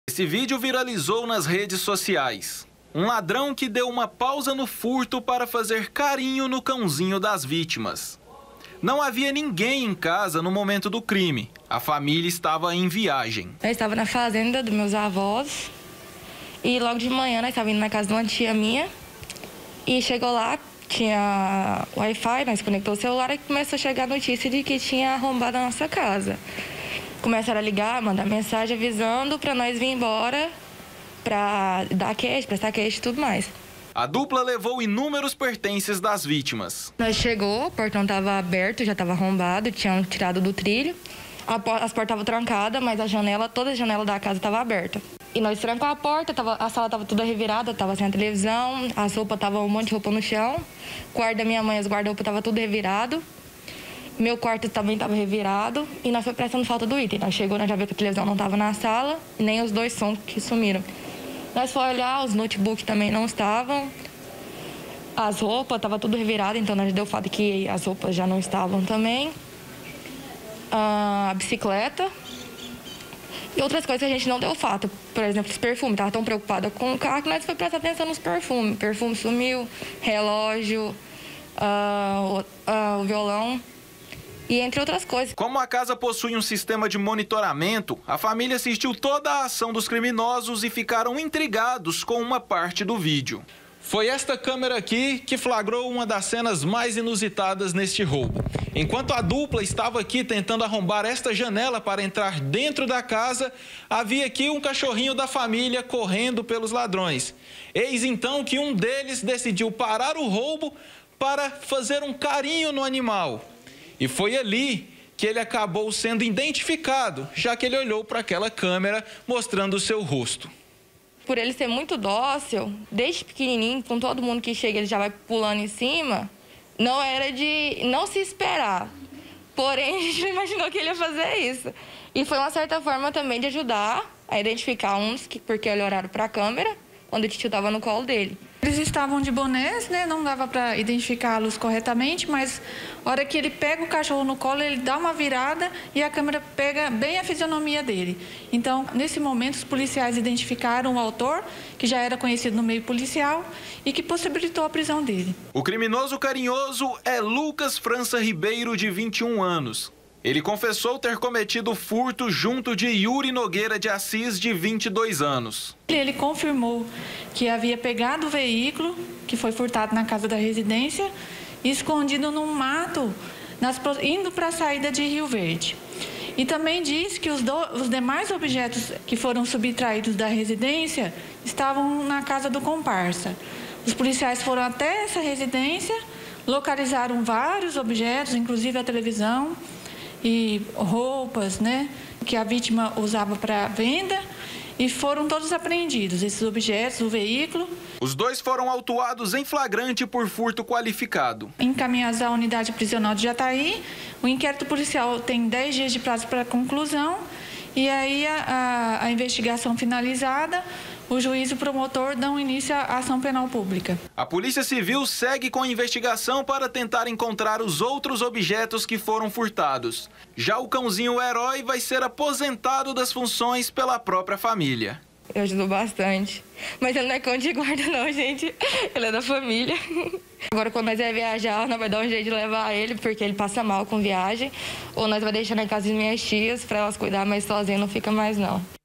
LADRÃO CARINHOSO: DONA DE CACHORRO SE SURPREENDEU COM O BANDIDO
NÓS MOSTRAMOS AQUI NO ÚLTIMO SÁBADO AS IMAGENS DE UM LADRÃO QUE FEZ CARINHO EM UM CACHORRO, ENQUANTO O COMPARSA DELE ARROMBAVA UMA CASA. NOSSA EQUIPE FOI ATÉ A CASA ONDE O CACHORRO MORA E CONVERSOU COM A DONA DO ANIMAL. ELA CONTA QUAL FOI A REAÇÃO DELA AO VER AS IMAGENS DE CIRCUITO DE SEGURANÇA.